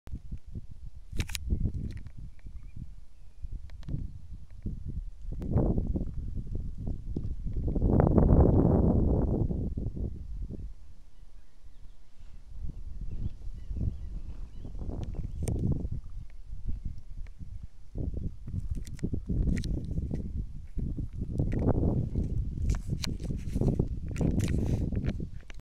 Vid Vägeröds Dalar 11 ‎april ‎2015 film 12
Lågvatten och man kan typ gå ända till Ängön.